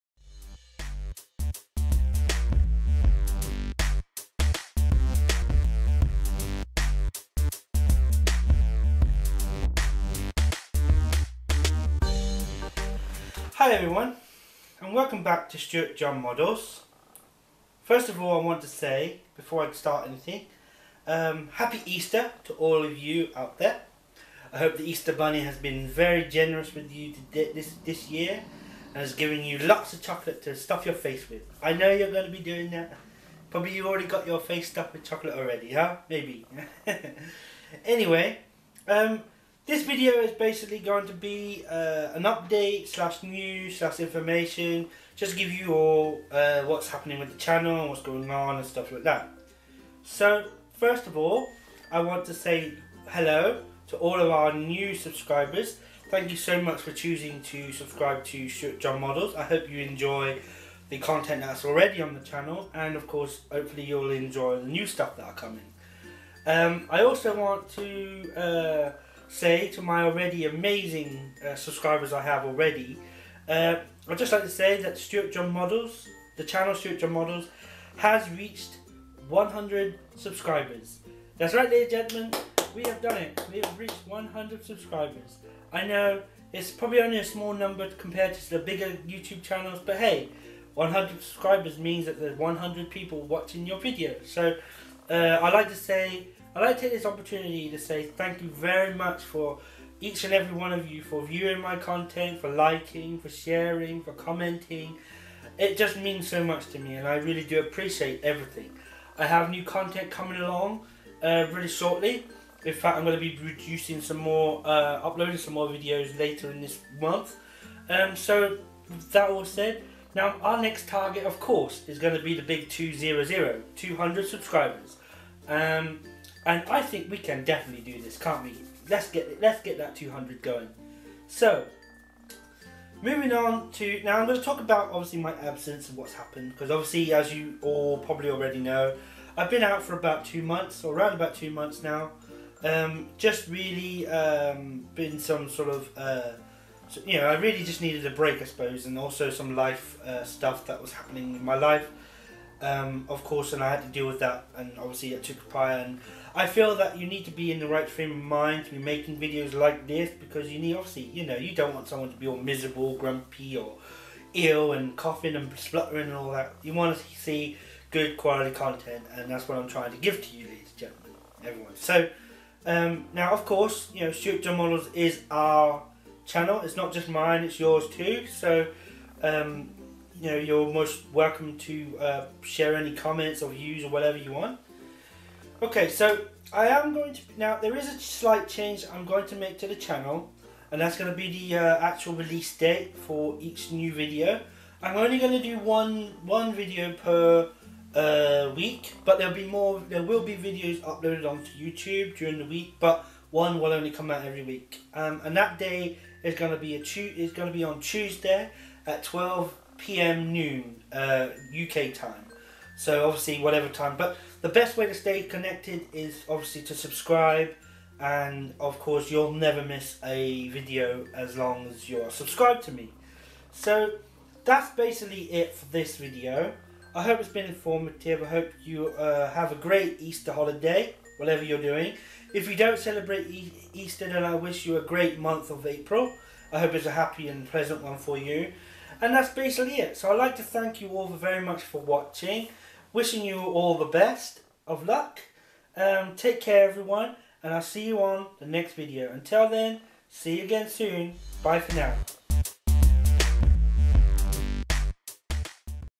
Stuart John Models Update/News/Info Video (Easter Special) (100 Subscribers Special)
Hi everyone. Welcome back to Stuart John Models. First of all I'd like to wish you all a very Happy Easter. :) I really hope that the Easter bunny has been kind to you this year with all that chocolate. :) This video is really a Update/News/Info to what is going on with our channel. So the first thing I want to do is say (HELLO) to all our new Subscribers. :) We welcome you to Stuart John Models I hope you will enjoy the new content I have coming up. Also to our already amazing Subscribers I thank you all for the support really could not do this without you. :D Okay so the big news is that our channel Stuart JohnModels has reached (100 Subscribers!) Wow thank you so much it's been a blast making these videos for you and myself and it's an amazing achievement. Of course we are not stopping there OH NO our next goal/target is (200 Subscribers) I KNOW WE CAN DO THIS. ;) Okay now I'm sure by now you all know that I've not been making any new video's of late for nearly 2 month's. I had a bit of time off really had a lot of stuff going on and not the best frame of mind to be making video's. But I am now back and already have new video's lined up. I am moving the new video day to every Tuesday at 12pm noon UK time. Going to be finishing the Classic Car Club and I do have new models to add.

HAPPY EASTER :) :D ;)